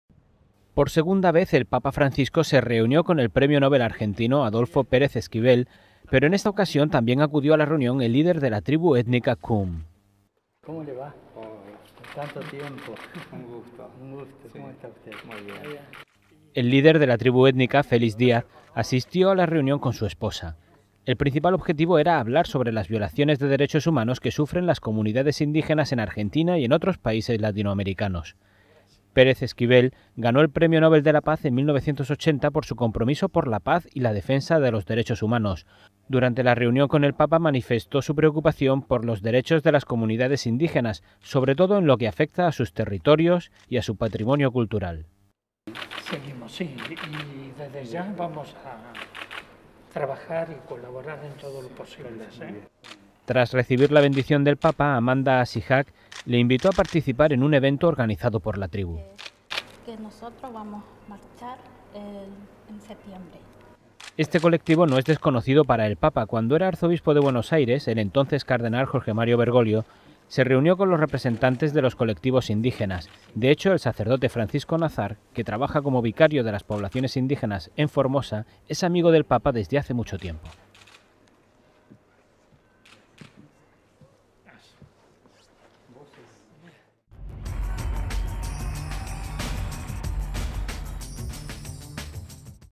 El Papa y el Nobel Pérez Esquivel defienden los derechos de las minorías indígenas
Por segunda vez el Papa Francisco se reunió con el premio Nobel argentino Adolfo Pérez Esquivel. Pero en esta ocasión también acudió a la reunión el líder de la tribu étnica Qom.